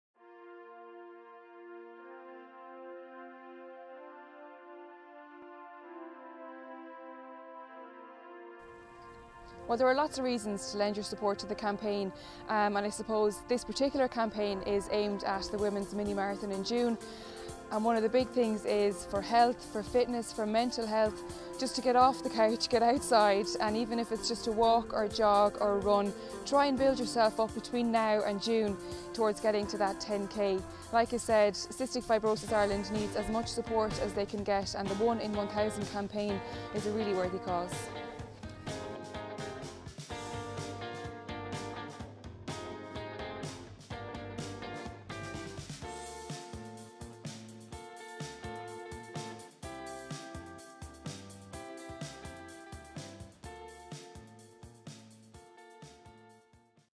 Be One in 1,000 with Evanne Ní Chuilinn
Join RTÉ Sports Presenter and One in 1,000 Ambassador Evanne Ní Chuilinn in supporting Cystic Fibrosis Ireland and register to take part in the VHI Women's Mini Marathon on Sunday 3rd June 2018.

The 10km route is suited to all levels of fitness so walk, jog or run - you set the pace. One person can make a difference, be the one person for Cystic Fibrosis Ireland and help make a difference to CF Care in Ireland. So gather your family, friends, colleagues and neighbours and take the first step with CFI.

or call Rachel on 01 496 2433 for more details.